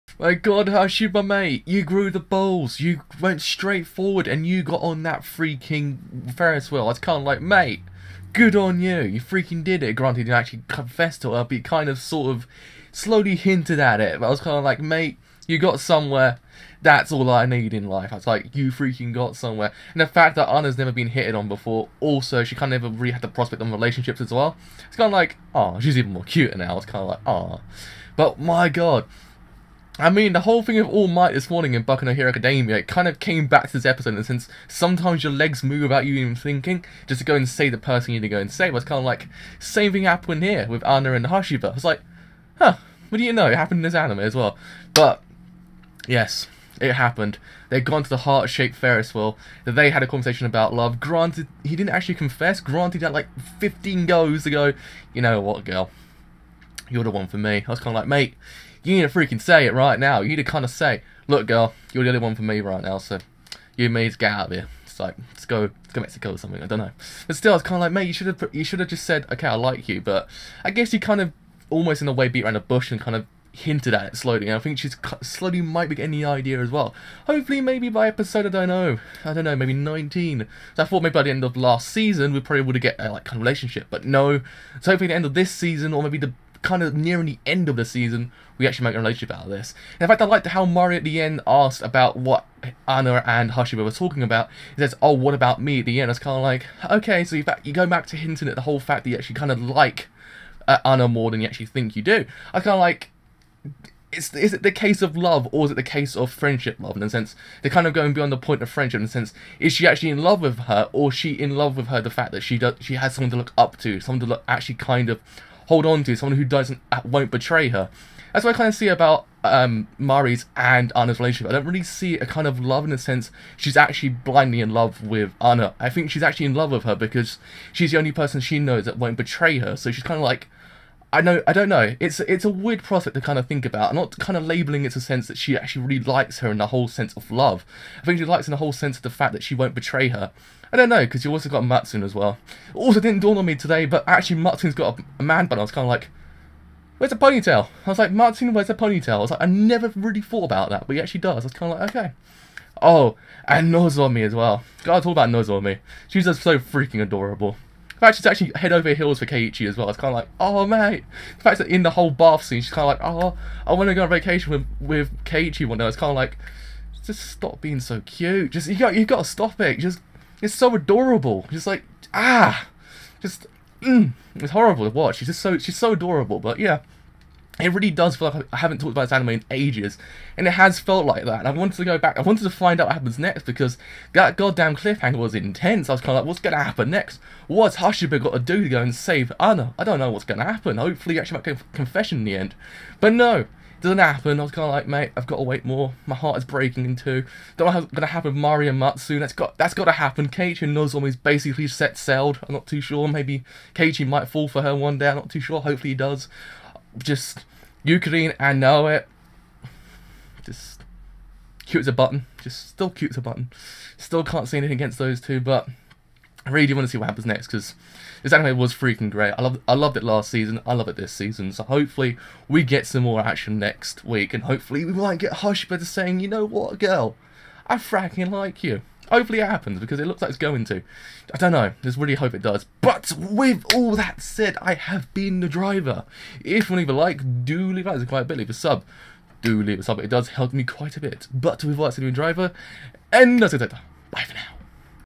Rainbow Days (虹色デイズ) Episode 13 Review/Reaction: Hashiba My Man
Welcome one and all to Rainbow Days Episode 13 where we finally get the answer to what happened last episode and Hashiba might have grown a little bit more as well :D

Can't get enough of my dulcet tones ?